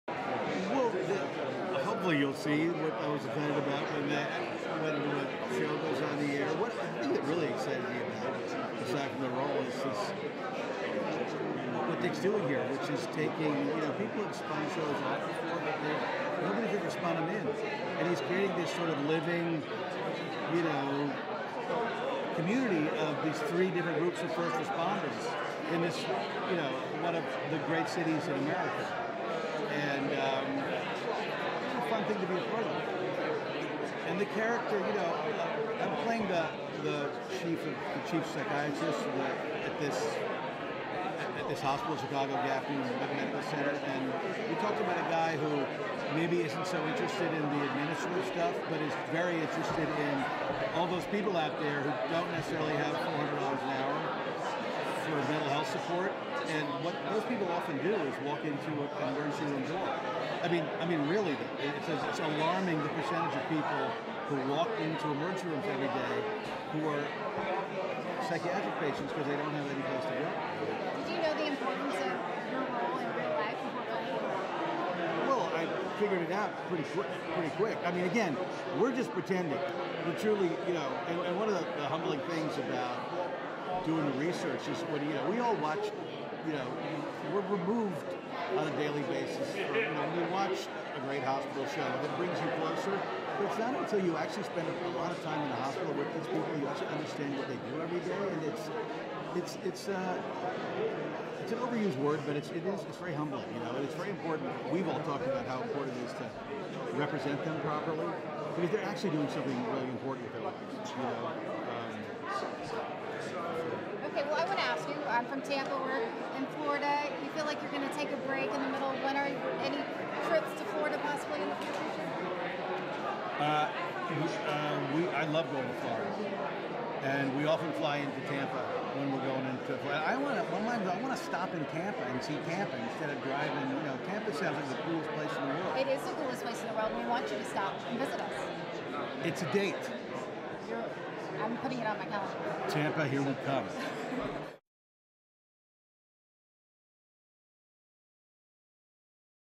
News Channel 8 talks with Chicago Med's Oliver Platt
Chicago Med airs on Tuesday nights.